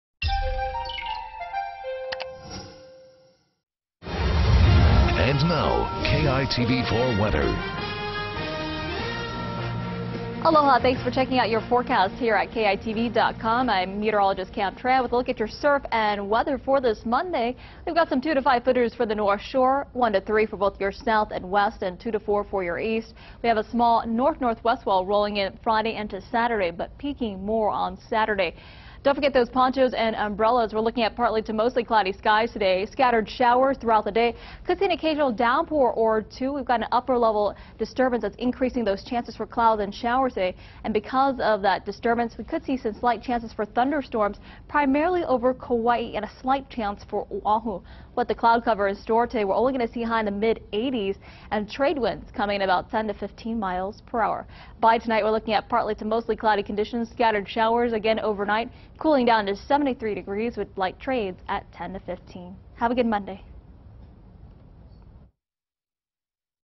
Monday morning weather forecast
Cam Tran has your Monday morning weather forecast. For news you need to know,